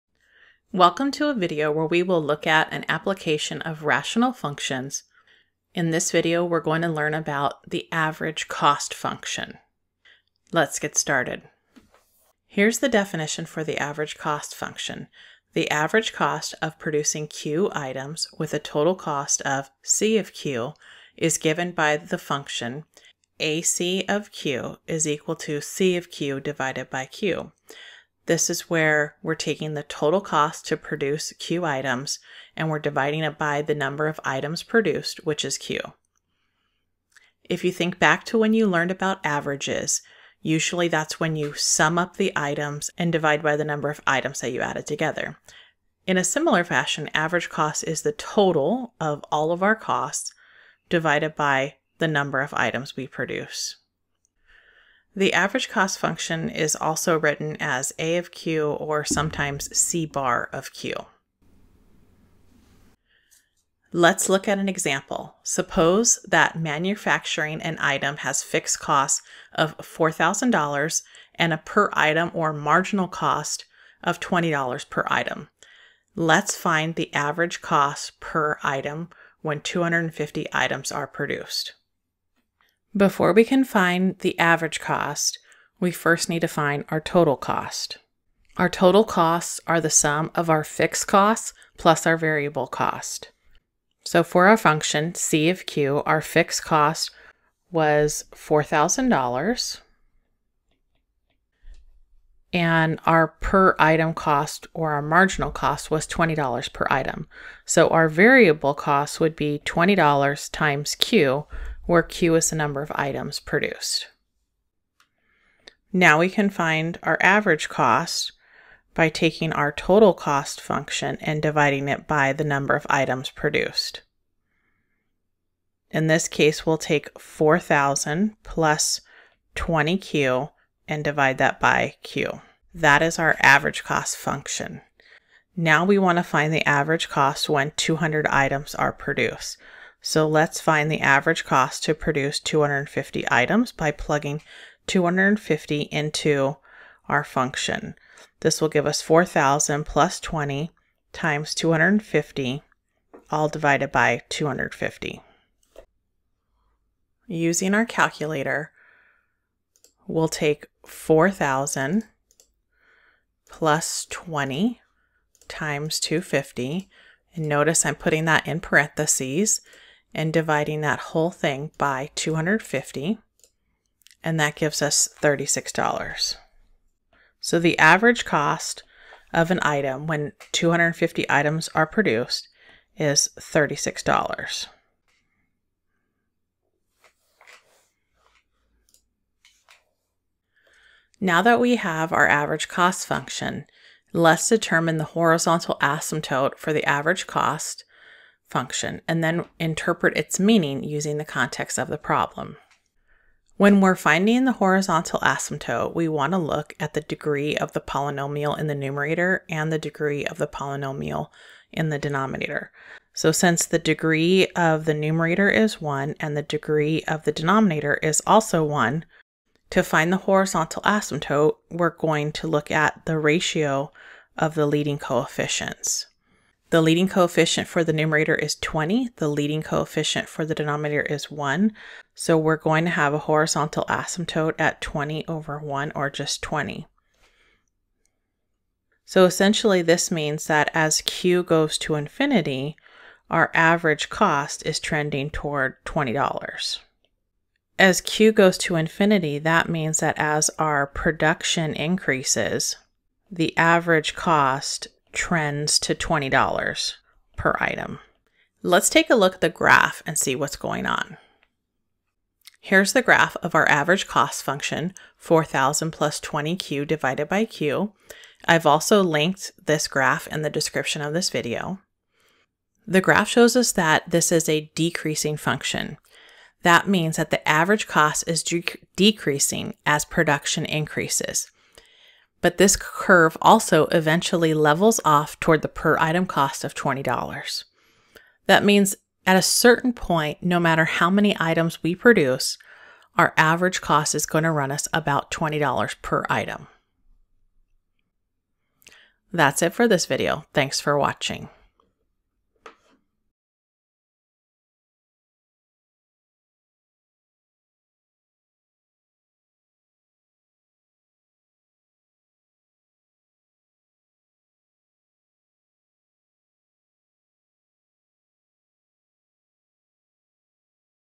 Applications of Rational Functions: Average Cost
In this video you can learn how we use rational functions to study the average cost to produce an item.

Click the link below to view the graph shared in this video using the free online graphing calculator website, Desmos.